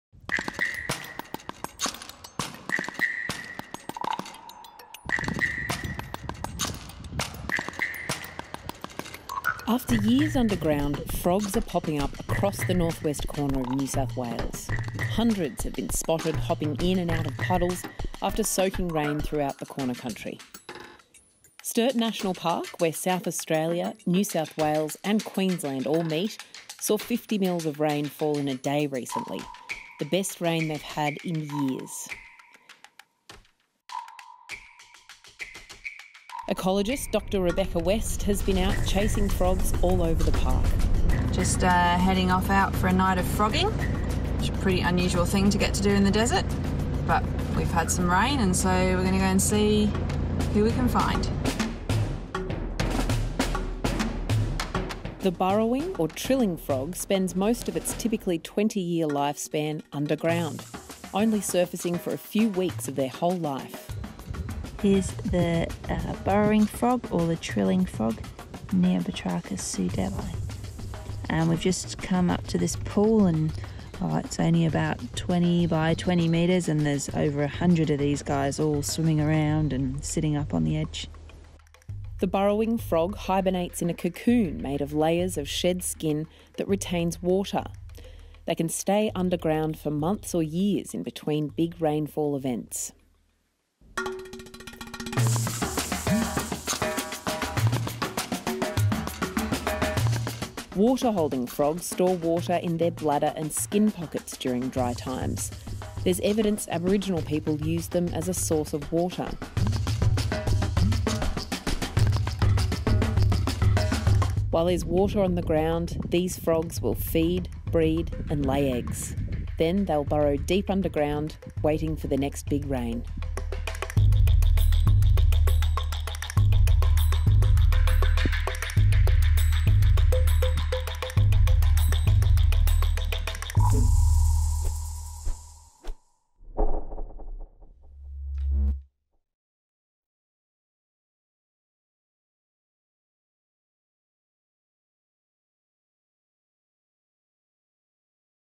Cute desert frogs emerge in the desert after big rains | ABC Australia
After years underground, different species of burrowing frogs are popping up across the north west corner of New South Wales.

From the Trilling frog that spends most of its life underground, cocooned in its own skin, to the Water-holding frog which was used as a source of water by Aboriginal people.

Hundreds have been spotted hopping in and out of puddles thanks to soaking rain throughout Sturt National Park, where South Australia, New South Wales and Queensland meet.

Once they’ve eaten their fill, mated and laid eggs, they’ll burrow back underground to wait for the next big rain.